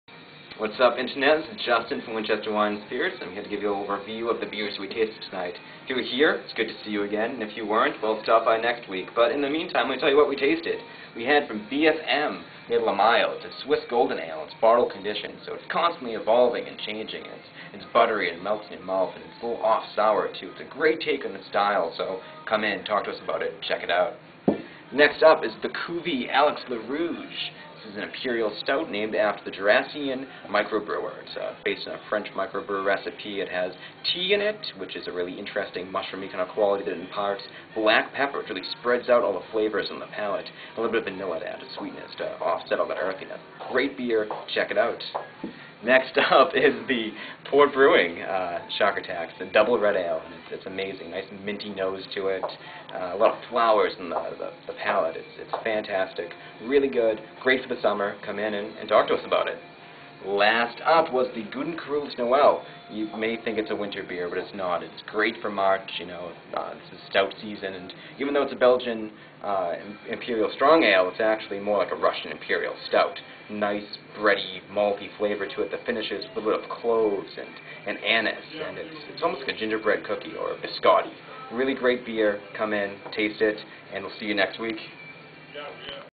winchester wine & spirits weekly beer tasting
Our Friday night beer tastings have really taken off!  Come on down and enjoy a complimentary brew with the Jedi & Winchester Wine & Spirits!